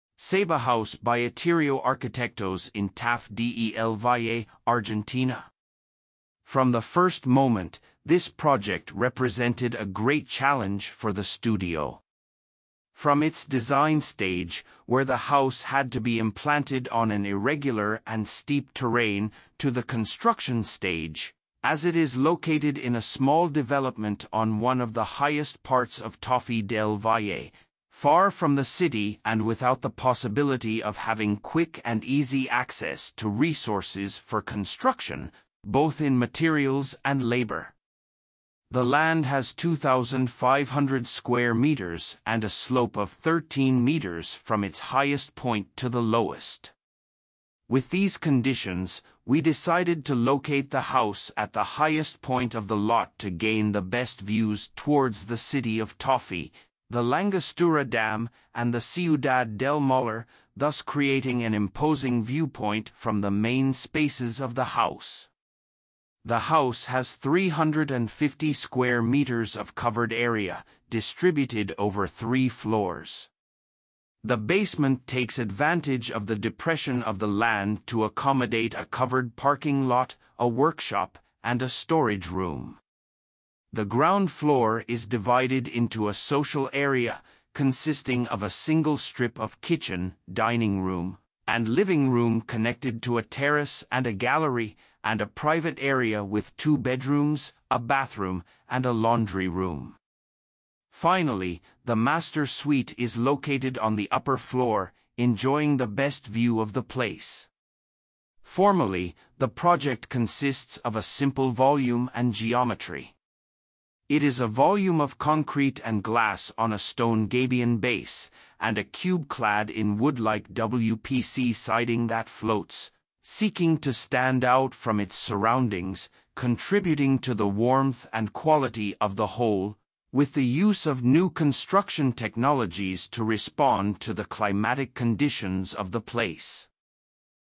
Seba House By Etéreo Arquitectos In TAFÍ DEL VALLE, ARGENTINA
From the first moment, this project represented a great challenge for the studio. From its design stage, where the house had to be implanted on an irregular and steep terrain, to the construction stage, as it is located in a small development on one of the highest parts of Tafí del Valle, far from the city and without the possibility of having quick and easy access to resources for construction, both in materials and labor.

Archi Studio

Architects: Etéreo Arquitectos
Area: 350 m²
Year: 2022
Photographs:Arq. Gonzalo Viramonte
Manufacturers:  EGGER, Aluar, Bronzer, Cemento Loma Negra, Cerro Negro, Destefano, FV, Ferrum S.A., Hafele, JOHNSON, Peirano, Roca, Tromen
Lead Architects: Santiago Vittar, Diego Madrid, Daniel Tello
Design Team: Emilia Martos, Isolda Elias, Franco Veneziano
Landscape: Lolo Nagore Paisajista
Collaborators: Salvador Ingrao
City: Tafí del Valle
Country: Argentina